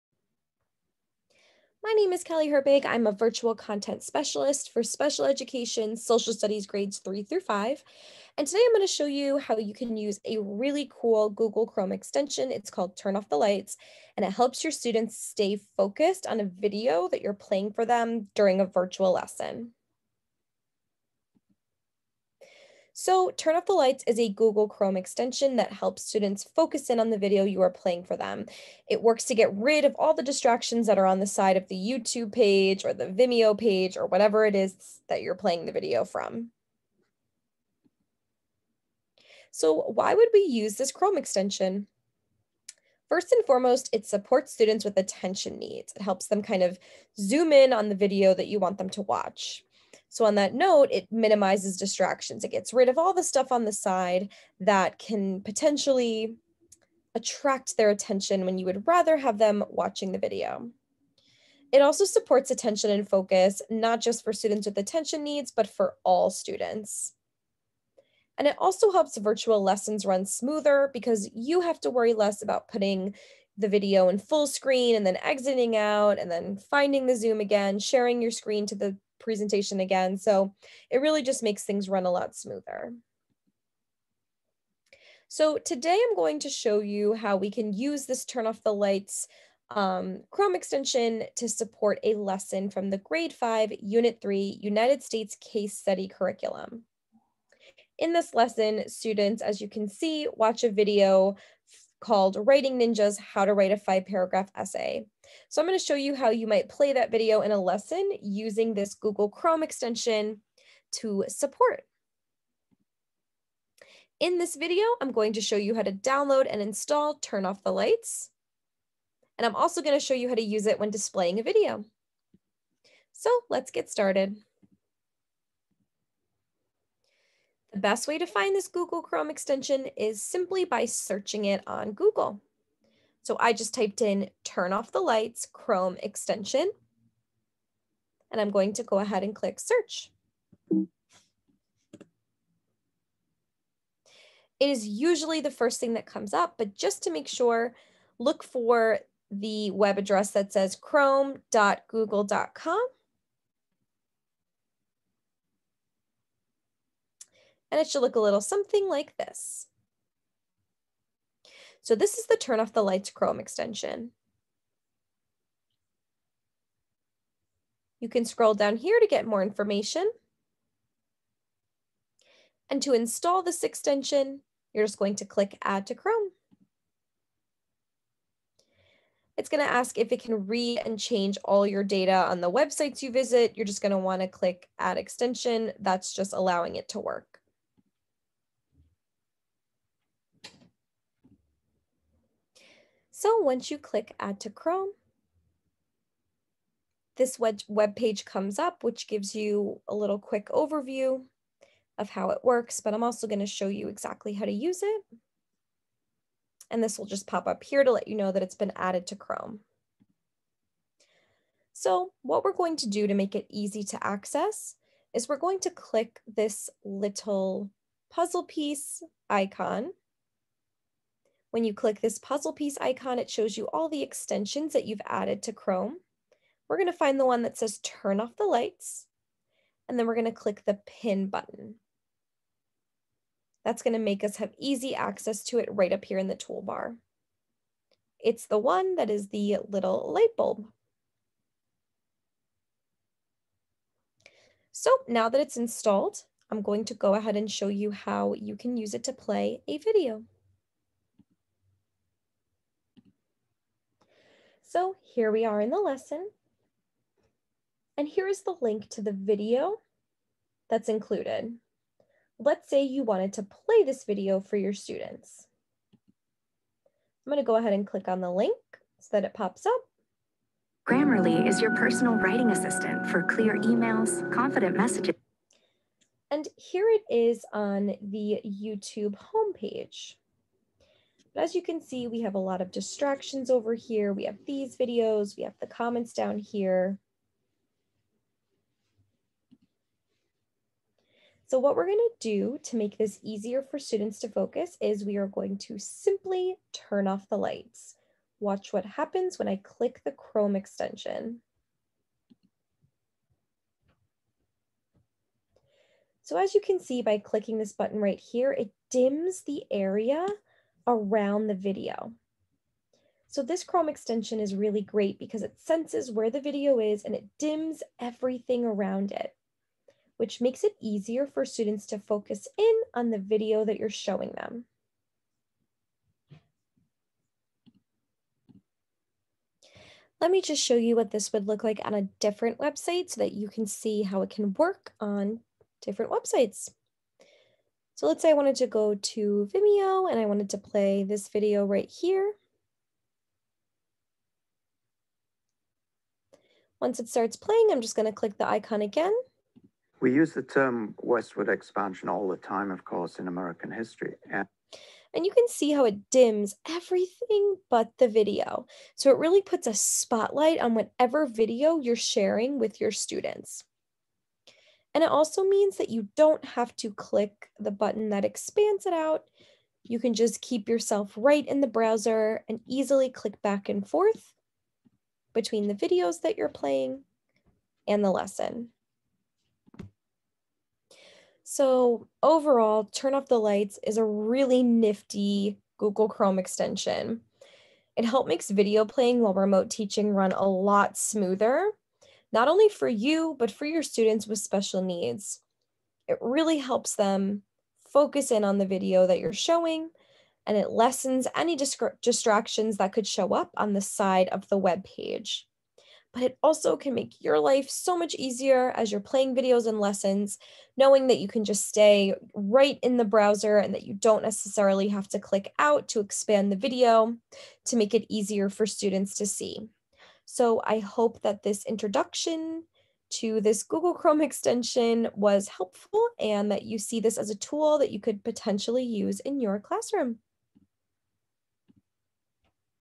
Using The Turn Off the Lights Chrome Extension
This video will teach you how to install and use the "Turn Off the Lights" Google Chrome extension.